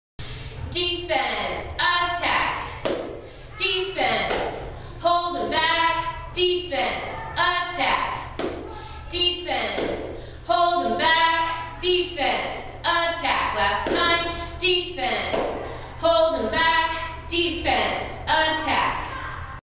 PSHS Cheer 2016 Tryout Defense Attack Cheer (Front View)
Call: Defense Attack!
[Clap]
Defense
[Clap]
Hold'em Back
Defense
[Pause]
Attack
[Repeat 2 more times]